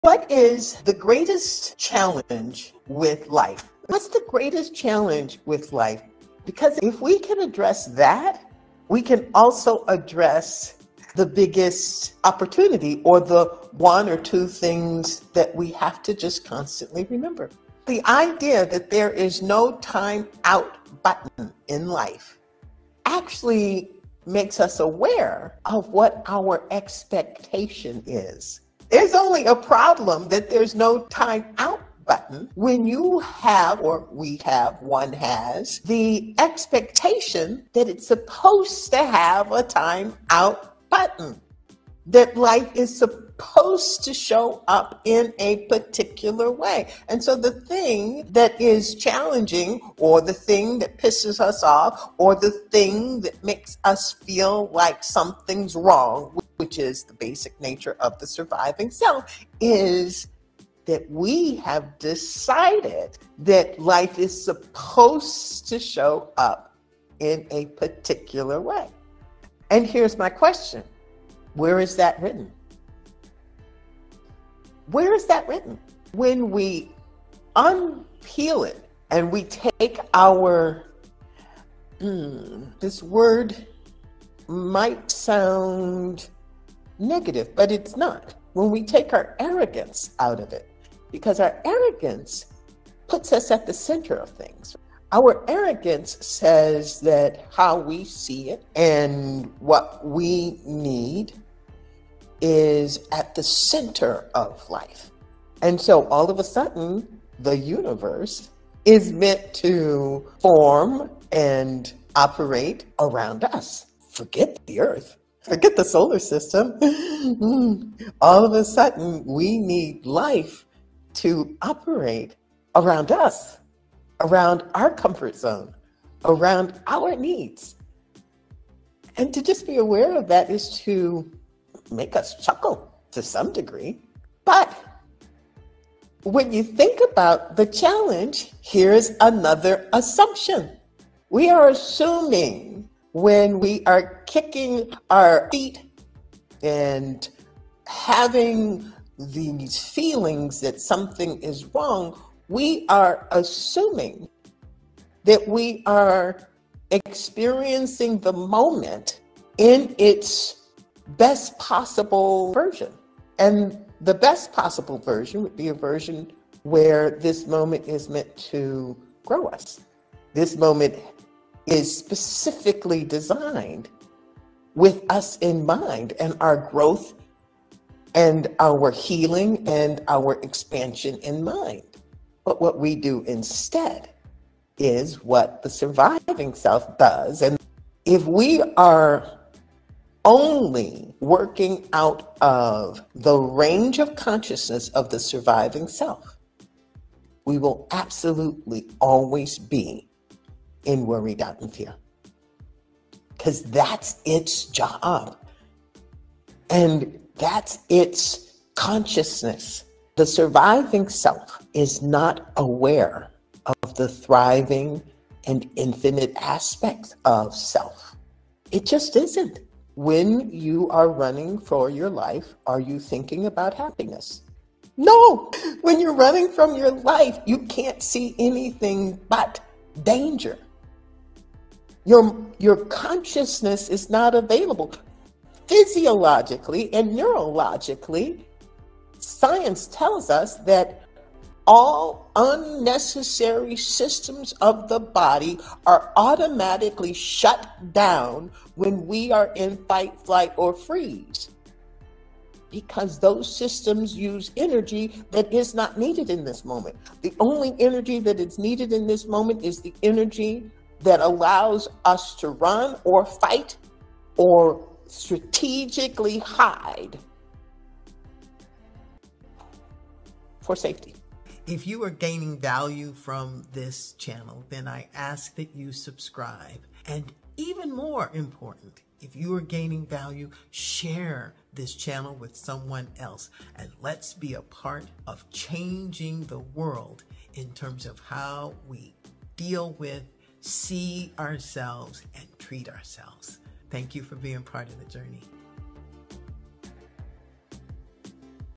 Focus on Your Thriving and Infinite Range of Self
Want to develop a habit that will change your life?

Get curious about the reactive surviving self that lives within you.

When you come to understand your true nature, realizing that you are more than your reactions and hurts, you unlock a transformative power that strengthens every area of your life. This video will expand your mind, and change your reality.